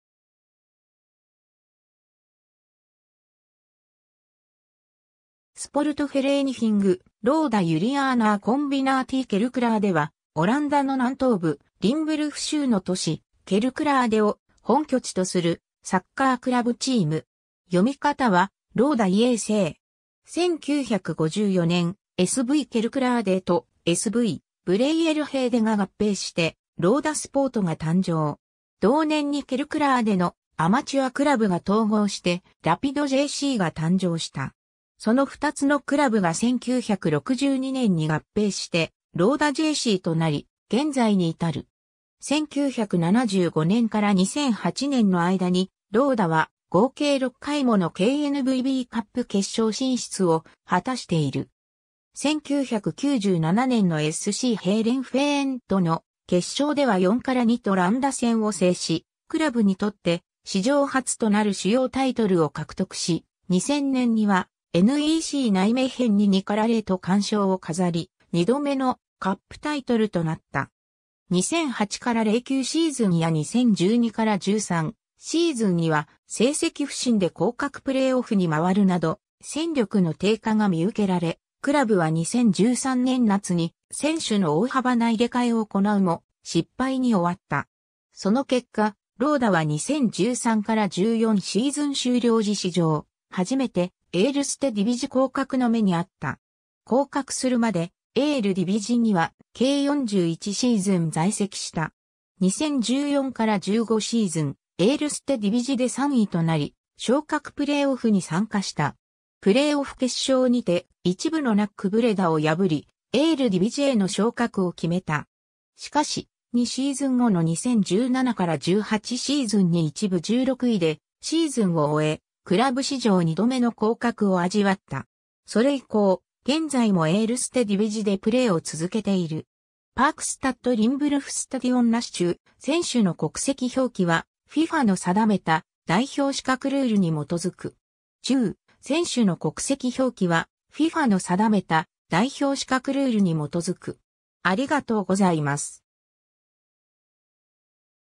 ローダJC
オランダ・リンブルフ州のスポーツ
スポルトフェレーニヒング・ローダ・ユリアーナー・コンビナーティー・ケルクラーデ（Sportvereniging Roda Juliana Combinatie Kerkrade (オランダ語発音: [ˈspɔrtfəˌreːnəɣɪŋ ˈroːdaː ˌjyliˈjaːnaː ˌkɔmbiˈnaː(t)si ˈkɛr(ə)kˌraːdə];)）は、オランダの南東部、リンブルフ州の都市ケルクラーデを本拠地とするサッカークラブチーム。
読み方は「ローダ・イェー・セー」。
 1954年、SVケルクラーデとSVブレイイェルヘイデが合併してローダ・スポートが誕生。
同年にケルクラーデのアマチュアクラブが統合してラピドJCが誕生した。
その2つのクラブが1962年に合併してローダJCとなり現在に至る。
 1975年から2008年の間にローダは合計6回ものKNVBカップ決勝進出を果たしている。
1997年のSCヘーレンフェーンとの決勝では4-2と乱打戦を制し、クラブにとって史上初となる主要タイトルを獲得し、2000年にはNECナイメヘンに2-0と完勝を飾り、2度目のカップタイトルとなった。
 2008-09シーズンや2012-13シーズンには成績不振で降格プレーオフに回るなど、戦力の低下が見受けられ、クラブは2013年夏に選手の大幅な入れ替えを行うも、失敗に終わった。
その結果、ローダは2013-14シーズン終了時史上初めてエールステ・ディヴィジ降格の目にあった。
降格するまで、エールディヴィジには計41シーズン在籍した。
 2014-15シーズン、エールステ・ディヴィジで3位となり、昇格プレーオフに参加した。
プレーオフ決勝にて、1部のNACブレダを破り、エールディヴィジへの昇格を決めた。
しかし、2シーズン後の2017-18シーズンに1部16位でシーズンを終え、クラブ史上2度目の降格を味わった。
それ以降、現在もエールステ・ディヴィジでプレーを続けている。
 パークスタット・リンブルフ・スタディオン なし 注：選手の国籍表記はFIFAの定めた代表資格ルールに基づく。